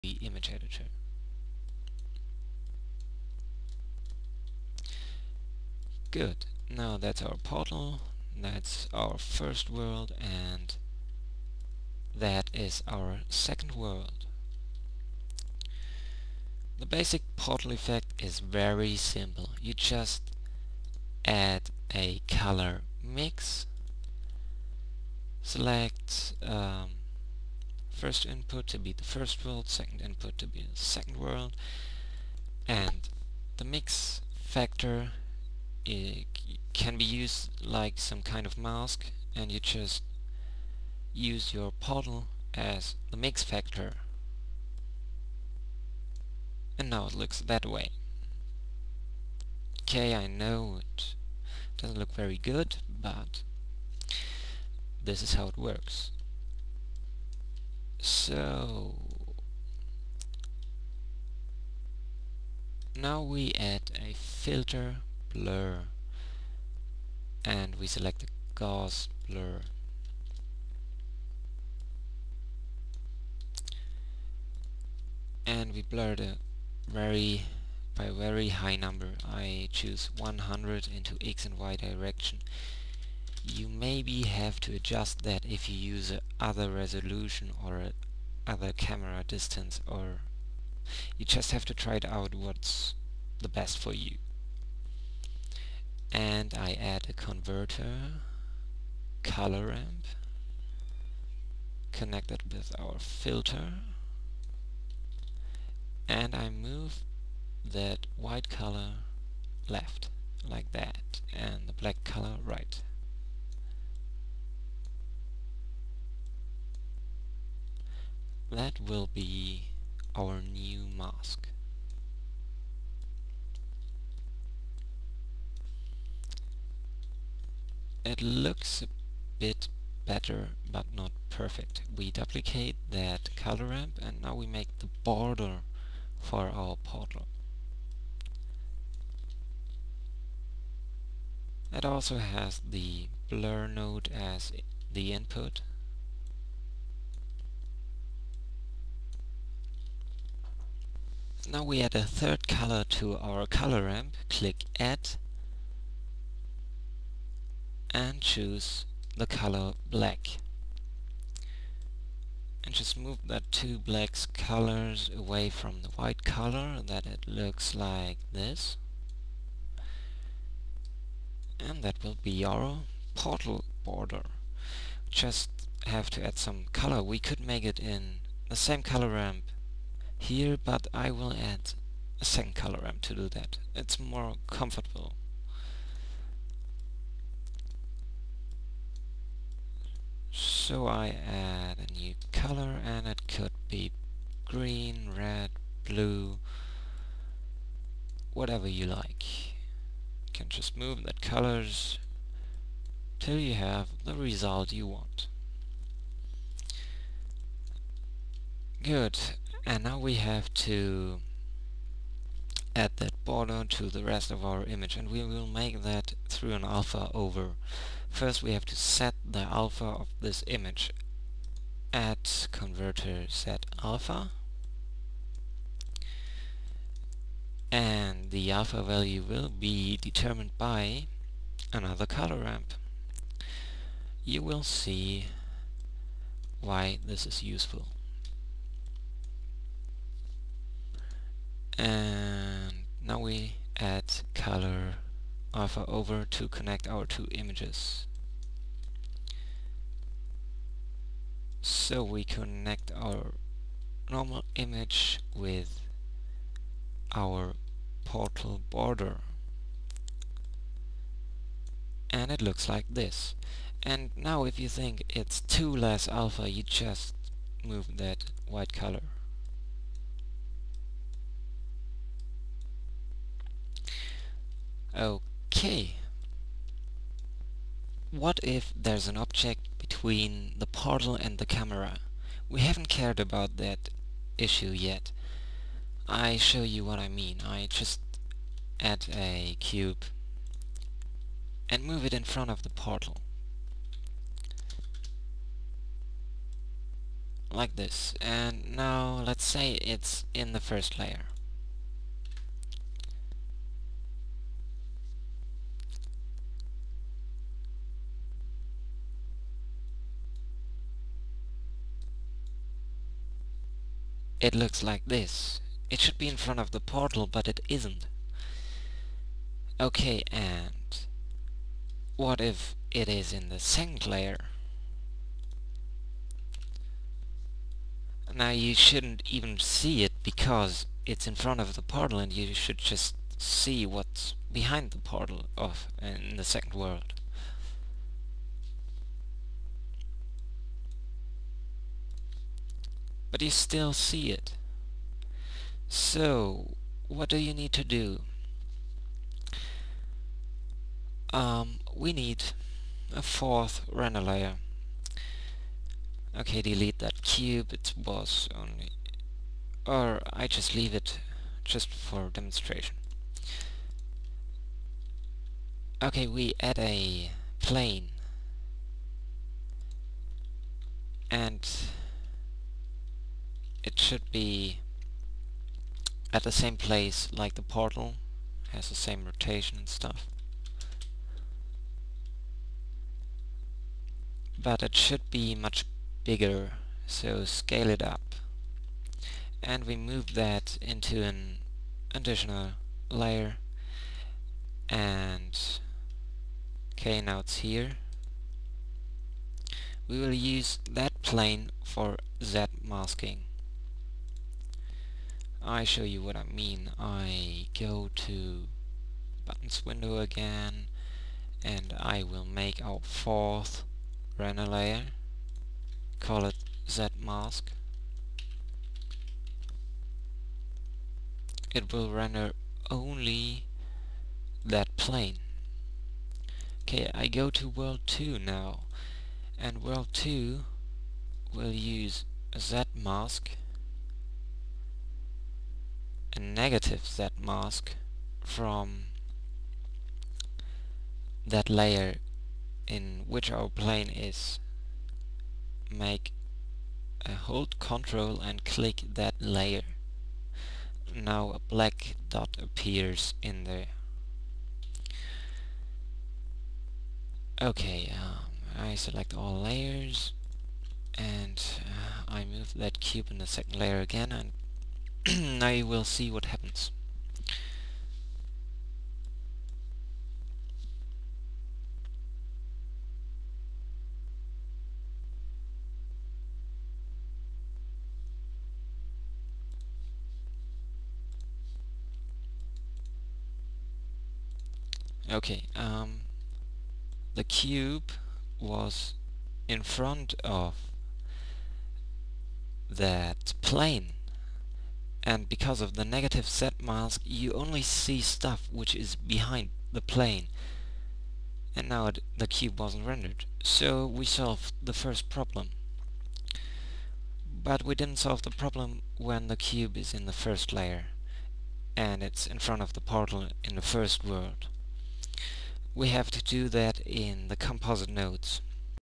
Nion's Blender Tutorial: Portal Effect pt. 2/3 Compositing and Masking
Blenderversion: 2.48a
A Tutorial on how to make a portal effect with Blender.